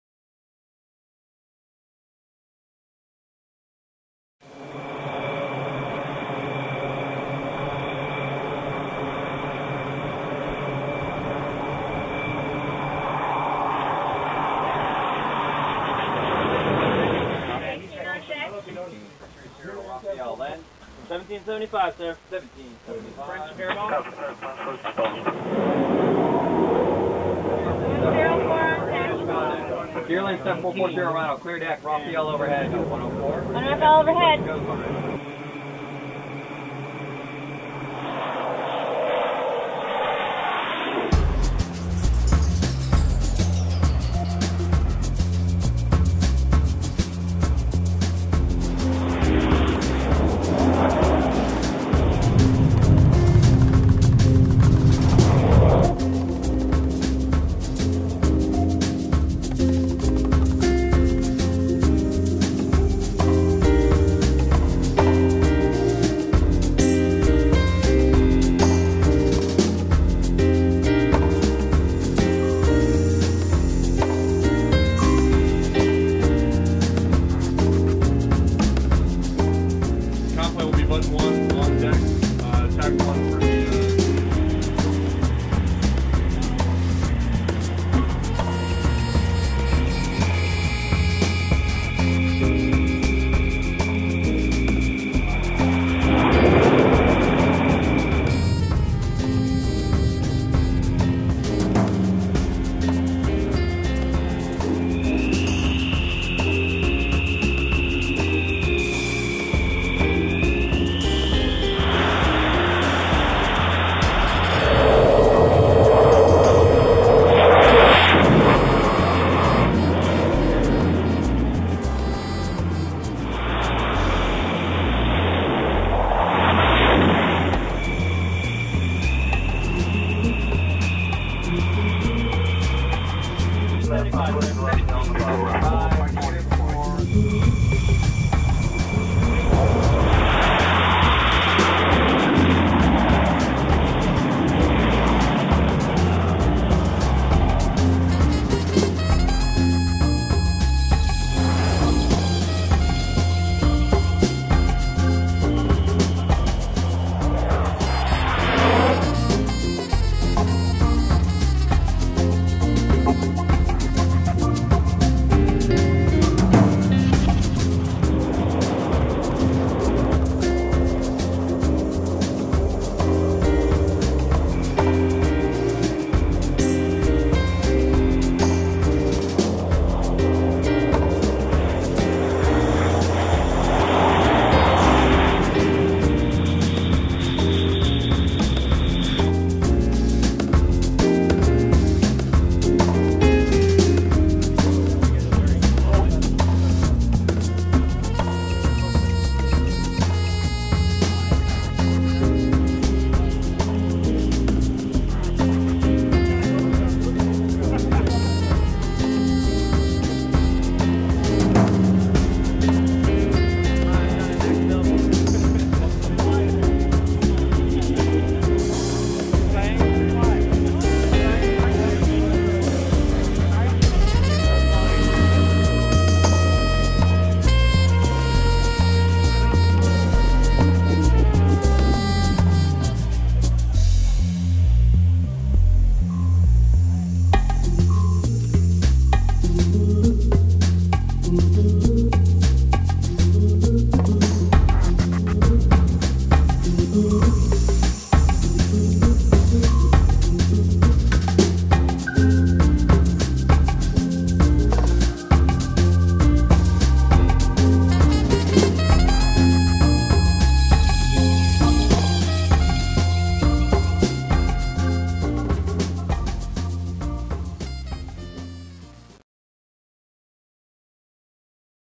RAFALE .France and America's - El portaaviones es el USS Theodore Roosevelt !
Julio de 2008  , entrenamiento Rafale y F-18/ Hornets